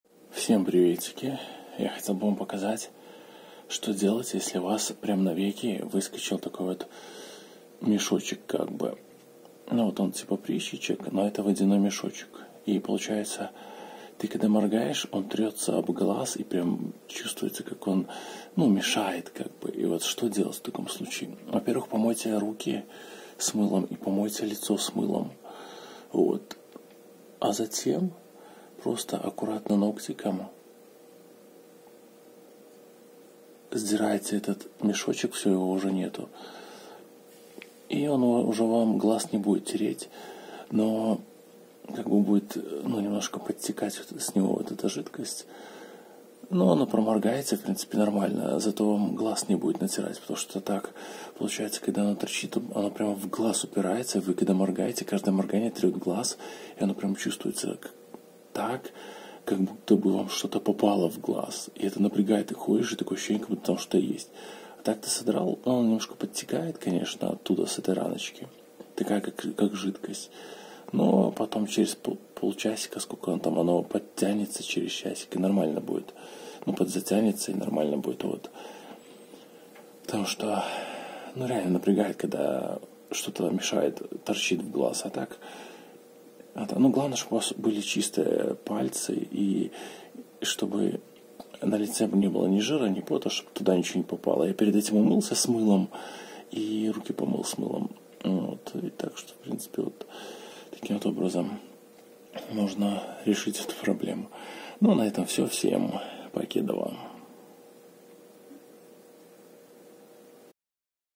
Водянистый прыщик ( пузырьки ) на веке глаза - что делать?
В этом видео, я покажу и расскажу как избавляюсь от водяных прыщиков на веках глаз.
Когда у меня такое случается, я так обычно и делаю.
Но в этот раз после того как записал это видео, почитал в интернете, и оказалось что это: секрет мейбомиевой железы. И врач рекомендует: Взять ватную палочку, смочить её глазными каплями Сульфацил натрия 20% и провести по краю века с прыщиком - он лопнет. Потом закапать в глаз Сульфацил натрия и всё. Таким образом вы предотвратите инфекционные заболевания глаз.